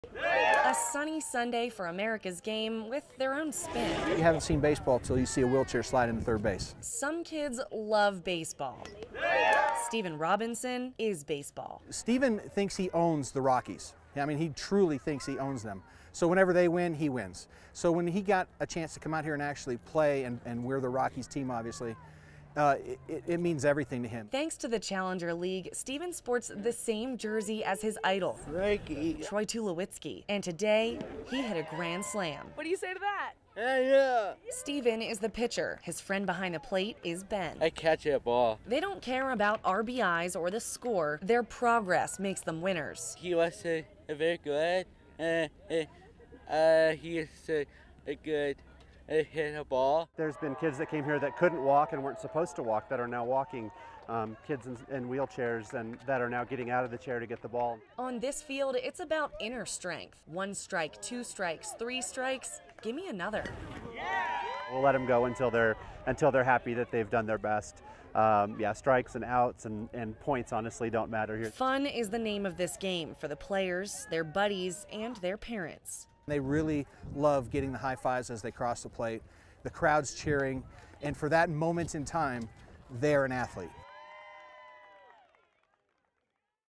A league of their own: the Challenger Baseball Division
Meet the players of the Colorado Springs Challenger Division, a group of athletes who would likely be unable to play on traditional little league teams due to physical or mental challenges.